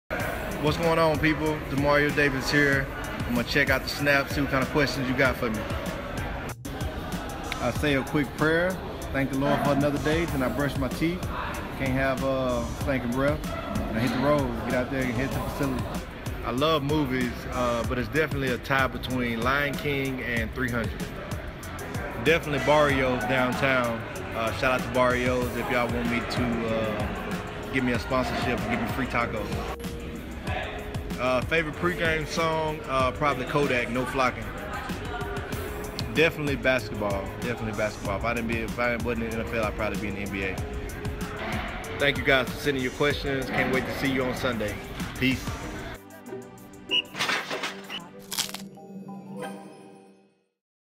Snack Chat with Demario Davis
Demario Davis answers some questions from the Dawg Pound in this week's Snack Chat.